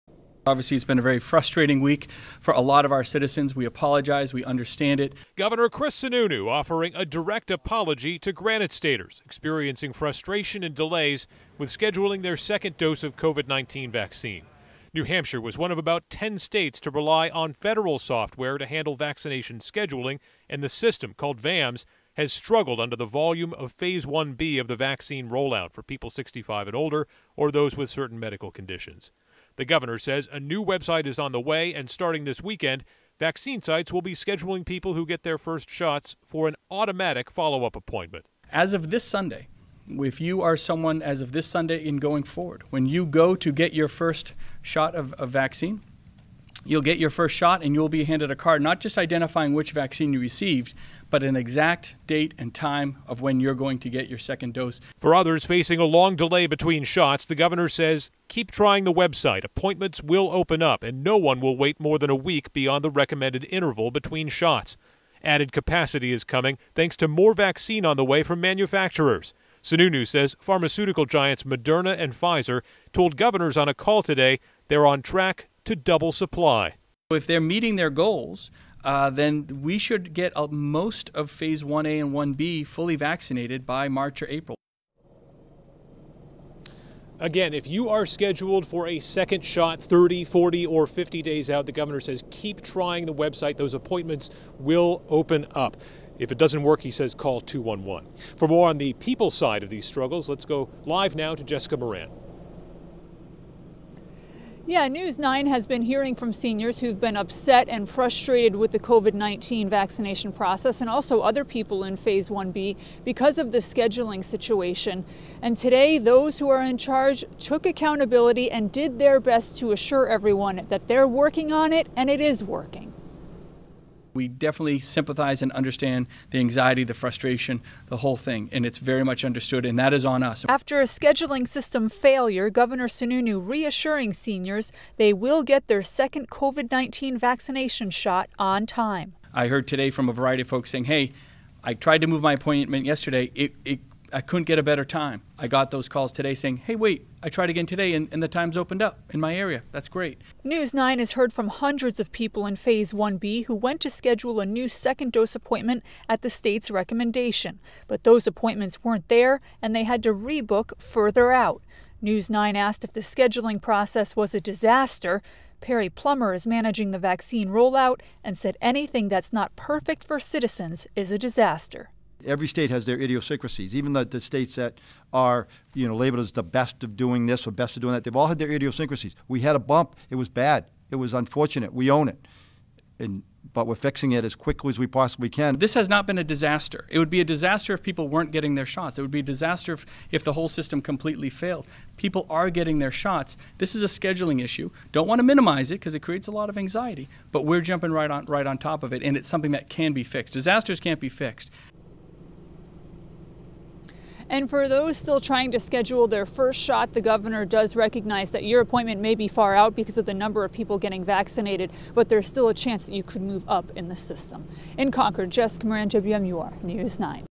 Governor apologizes for 2nd shot scheduling delays
Gov. Chris Sununu and other state officials on Thursday acknowledged serious problems some Granite Staters have had scheduling their second COVID-19 vaccinations and said they were working to fix the problem.